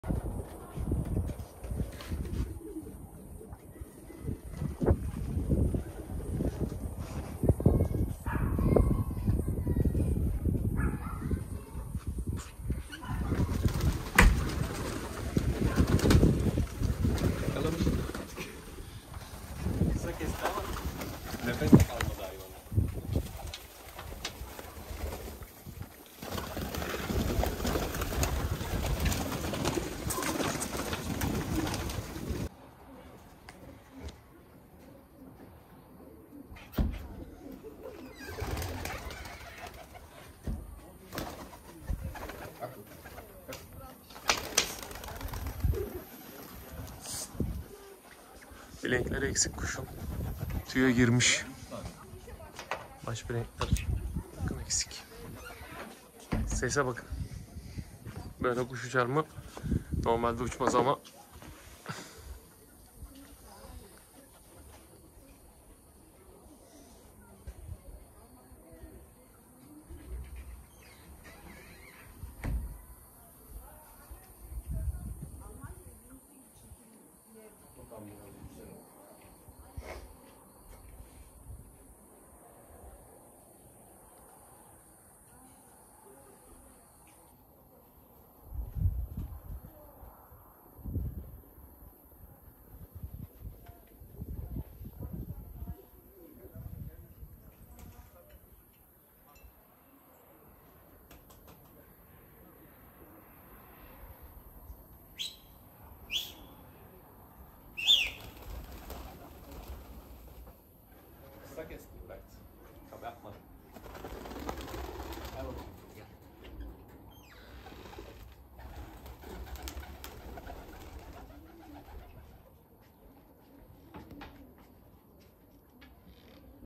✅ DİDİM ⚓️SERHAT KAPTAN⚓️Dönek uçurdu.Spinner pigeons.Dunek Dönek güvercin dönüş göbeği parlatıyor.
✅ DİDİM ⚓️SERHAT KAPTAN⚓️Dönek uçurdu.Spinner pigeons.Dunek Dönek güvercin dönüş.